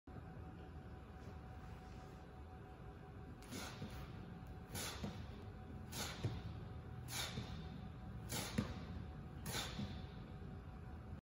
Power Push Up with Med Ball
Complete a plyo/power push up and switch which hand is on the med ball while in the air.